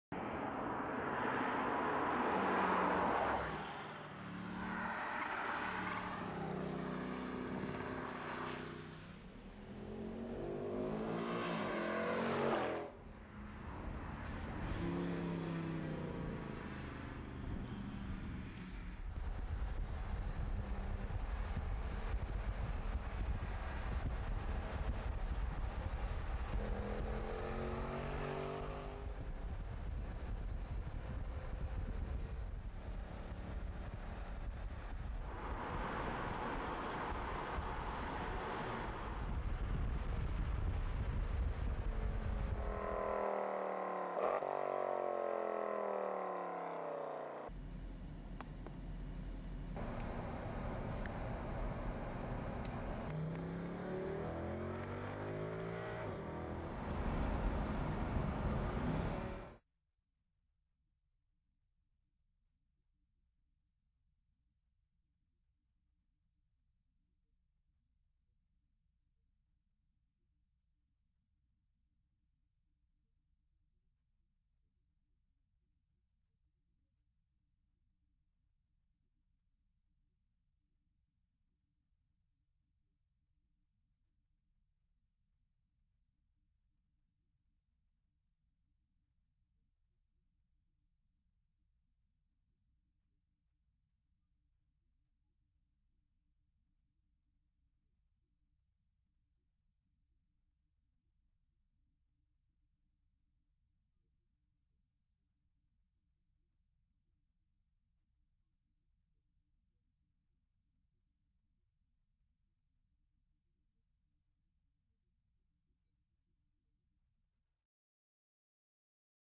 2009 Mercedes Benz SL63 AMG.
Running footage of 2009 Mercedes Benz SL63 AMG.